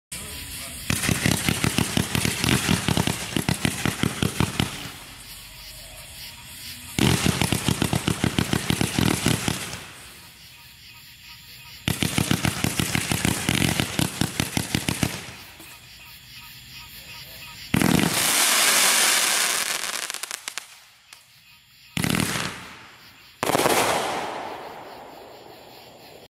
Neon Peacock
Neon Peacock is a 500 gram 93 multishot cake made by blackcat fireworks